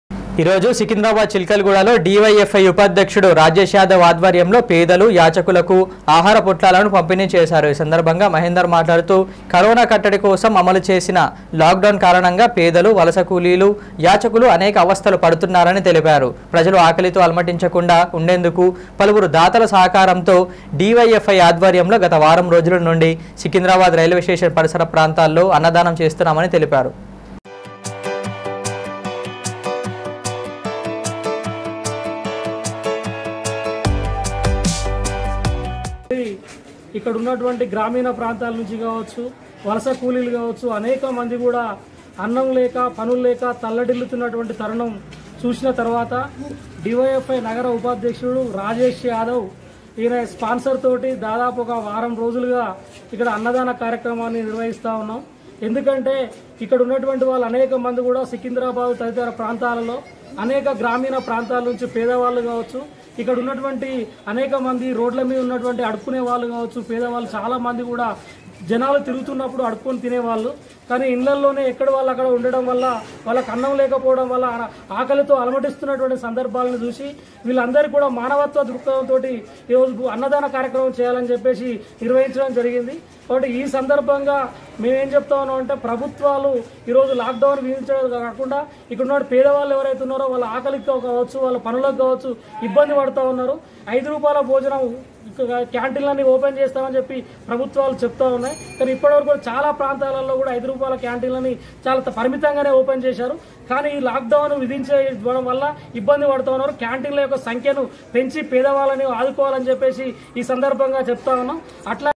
సికింద్రాబాద్ చిలుకలగూడ లో డి వై ఎఫ్ ఐ ఆధ్వర్యంలో ఆహార పొట్లాల పంపిణీ | Ma Telangana
సికింద్రాబాద్ చిలుకలగూడ లో  డి వై ఎఫ్ ఐ ఆధ్వర్యంలో ఆహార పొట్లాల పంపిణీ